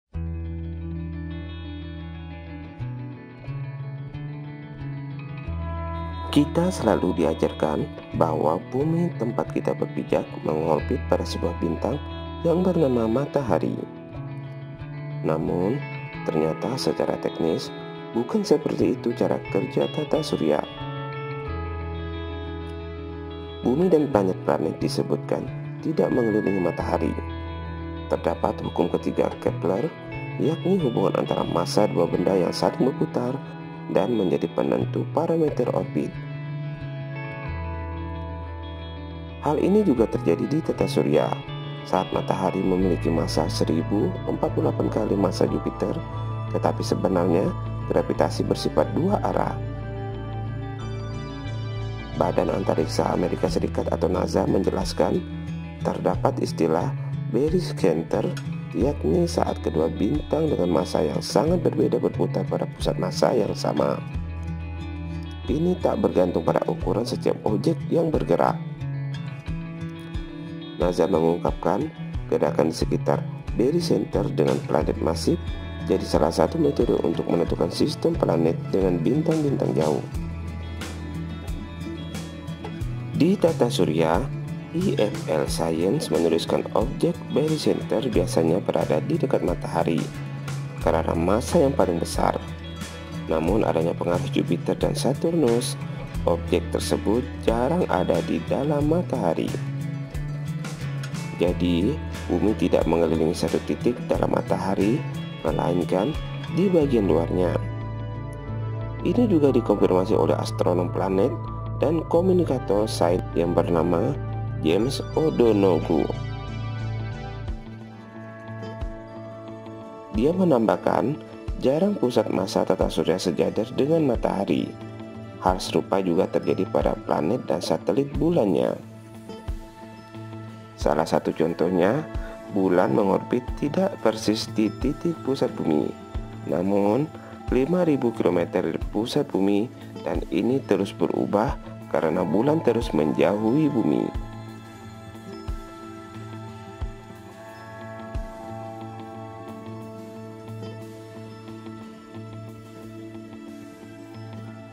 FAKTA BARU !! NASA, BUMI TAK LAGI MENGELILINGI MATAHARI
Bumi dan planet-planet disebutkan tidak mengelilingi Matahari. Terdapat hukum ketiga Kepler yakni hubungan antar massa dua benda yang saling berputar dan menjadi penentu parameter orbit.

NASA menjelaskan terdapat istilah barycenter yakni saat kedua bintang dengan massa yang sangat berbeda berputar pada pusat massa yang sama. Ini tak bergantung pada ukuran setiap objek yang bergerak.

"Gerak di sekitar barycenter dengan planet masif jadi salah satu metode untuk menemukan sistem planet dengan bintang-bintang jauh," jelas NASA.

Di Tata Surya, IFL Science menuliskan objek barycenter biasanya berada di dekat Matahari karena massa yang paling besar. Namun adanya pengaruh Jupiter dan Saturnus, objek tersebut jarang ada di dalam Matahari.

Jadi Bumi tidak mengelilingi satu titik dalam Matahari, melainkan di bagian luarnya. Ini juga dikonfirmasi oleh astronom planet dan komunikator sains bernama James O'Donoghue.